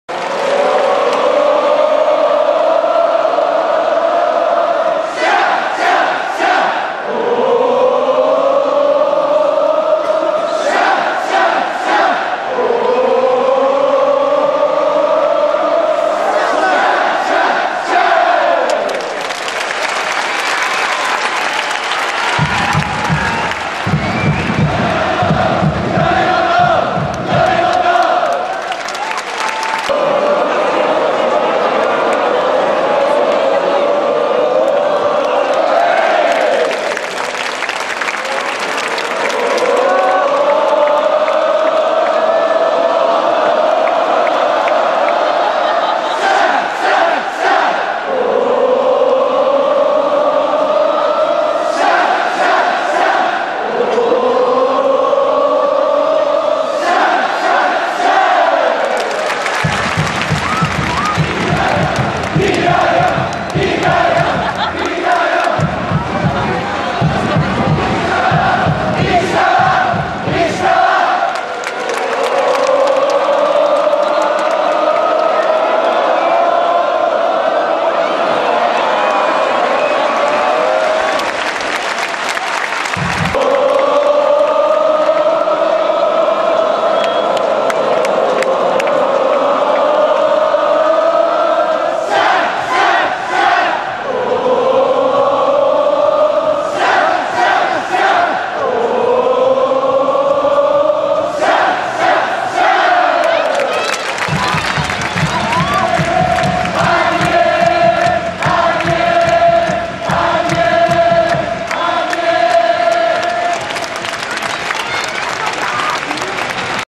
FC東京、米本、平山、羽生、ナビスコ準々決勝、名古屋、シャー
2009年7月15日に味の素スタジアムで行われた、ナビスコカップ準々決勝第1戦　ホーム名古屋戦での米本拓司と平山相太と羽生直剛のシャー